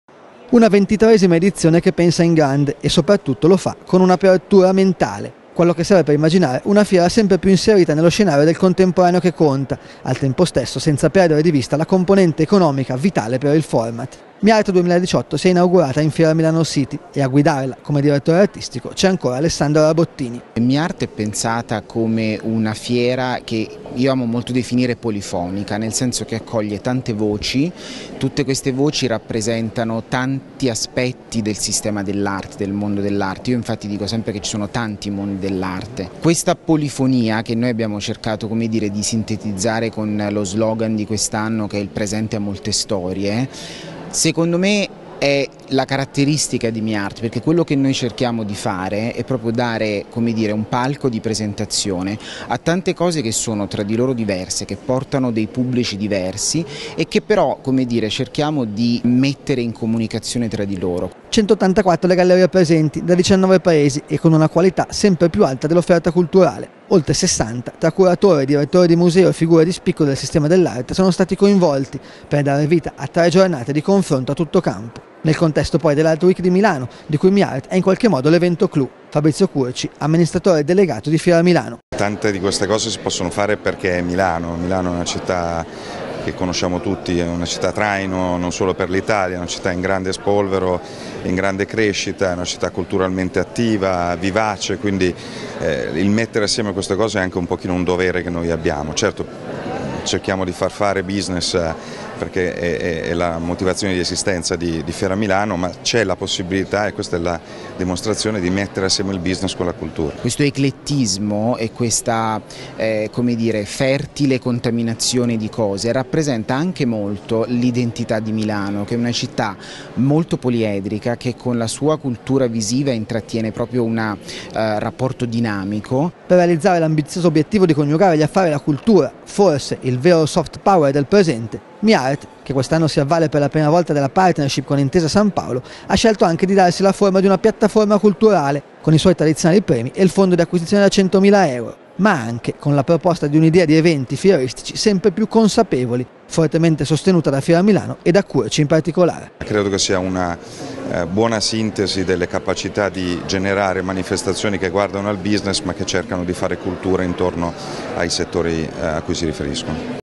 miart 2018: arte, polifonia e cultura dialogano con il business
Milano (askanews) - Una ventitreesima edizione che pensa in grande e, soprattutto, lo fa con apertura mentale. Quella che serve per pensare una fiera sempre più inserita nello scenario del contemporaneo che conta senza, al tempo stesso, perdere di vista la componente economica vitale per il format. miart 2018 si è inaugurata in Fieramilanocity e a guidarla, come direttore artistico, c'è ancora Alessandro Rabottini.
"miart - ha detto ad askanews - è pensata come una fiera che io amo molto definire polifonica, nel senso che accoglie tante voci, tutte queste voci rappresentano diversi aspetti del sistema dell'arte. Io infatti dico sempre che ci sono tanti mondi dell'arte. Questa polifonia, che noi abbiamo cercato di sintetizzare con lo slogan di quest'anno che è 'Il presente ha molte storie', secondo me è la caratteristica di miart. Perché quello che noi cerchiamo di fare è dare un palco di presentazione a tante cose che sono tra loro diverse, che portano dei pubblici diversi, e che però cerchiamo di mettere in comunicazione tra di loro".
184 le gallerie presenti, da 19 Paesi e con una qualità sempre più alta dell'offerta culturale. Oltre 60 tra curatori, direttori di museo e figure di spicco del sistema dell'arte sono stati coinvolti, per dare vita a tre giornate di confronto a tutto campo. Nel contesto poi della Art week di Milano, di cui miart è in qualche modo l'evento clou.
Fabrizio Curci, amministratore delegato di Fiera Milano: "Tante di queste cose - ci ha spiegato - si possono fare perché siamo a Milano, una città che conosciamo tutti, una città che fa da traino non solo per l'Italia, una città in grande spolvero e grande crescita, culturalmente attiva, vivace, quindi il mettere insieme queste cose è anche un dovere che noi abbiamo. Certo cerchiamo di far fare business, perché è la motivazione dell'esistenza di Fiera Milano, però c'è la possibilità, e questa è la dimostrazione, di mettere insieme il business e la cultura".
"Questo eclettismo e questa fertile contaminazione di cose - gli ha fatto eco Rabottini - rappresentano anche molto l'identità di Milano, una città molto poliedrica che con la sua cultura visiva intrattiene proprio un rapporto dinamico".
Per realizzare l'ambizioso obiettivo di coniugare gli affari e la cultura - forse il vero soft power del presente - miart, che quest'anno si avvale per la prima volta della partnership con Intesa Sanpaolo, ha scelto anche di darsi la forma di una piattaforma culturale, con i suoi tradizionali premi e il fondo di acquisizioni da 100mila euro, ma anche con la proposta di un'idea di eventi fieristici sempre più "consapevoli", fortemente sostenuta da Fiera Milano e da Curci in particolare.
"Credo che sia una buona sintesi - ha concluso l'ad - delle capacità di generare manifestazioni che guardano al business, ma che cercano di fare cultura intorno ai settori a cui si riferiscono".